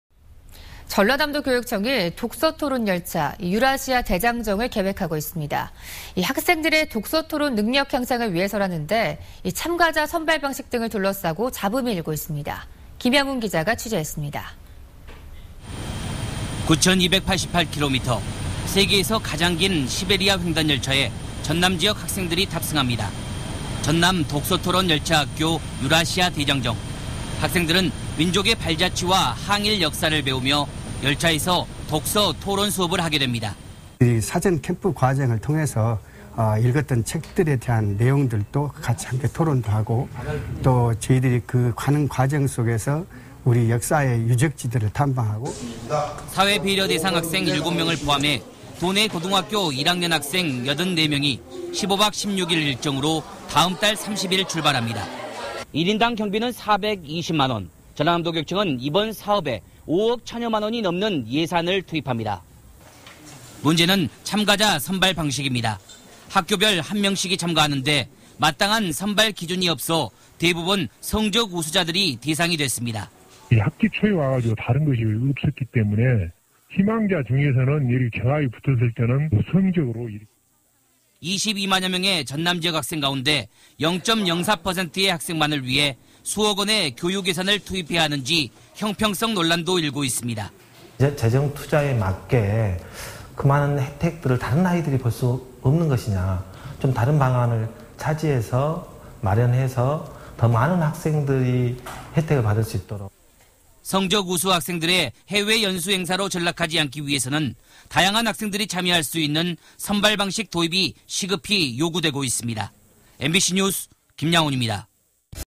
[뉴스투데이]성적 위주 선발방식 바꿔야
◀앵 커▶

전라남도교육청이 독서 토론 열차
유라시아 대장정을 계획하고 있습니다.

학생들의 독서토론 능력 향상을 위해서라는데
참가자 선발방식 등을 둘러싸고 잡음이 일고
있습니다.

김양훈 기자가 취재했습니다.

9288km 세계에서 가장 긴 시베리아 횡단열차에
전남지역 학생들이 탑승합니다.

전남 독서토론 열차학교 유라시아 대장정..

학생들은 민족의 발자취와 항일 역사를 배우며
열차에서 독서,토론 수업을 하게 됩니다.

◀인터뷰▶ 박석주 장학관
책에 대해서 토론하고 가는 과정 속에서
역사 유적지를 탐방하고.//

사회배려 대상 학생 7명을 포함해 도내
고등학교 1학년 84명이 15박 16일 일정으로
다음 달 30일 출발합니다.(투명CG)

s/u 1인당 경비는 420만 원, 전라남도교육청은
이번 사업에 5억천여만 원이 넘는 예산을
투입합니다.(투명CG)

문제는 참가자 선발 방식입니다.

학교별 1명씩이 참가하는데 마땅한 선발 기준이
없어 대부분 성적 우수자들이 대상이 됐습니다.

◀녹 취▶ 학교 관계자
/학기 초에 (공문이)와서 (기준이) 없기 때문에
희망자간에 경합이 붙으면 성적으로 뽑았죠./

22만여 명의 전남지역 학생 가운데
0.04%의 학생만을 위해 수억 원의 교육예산을
투입해야하는지 형평성 논란도 일고 있습니다.

◀인터뷰▶ 정관호/전교조
더 많은 학생이 혜택을 보도록 해야 합니다.//

성적우수 학생들의 해외연수 행사로 전락하지 않기 위해서는 다양한 학생들이 참여할 수 있는
선발방식 도입이 시급히 요구되고 있습니다.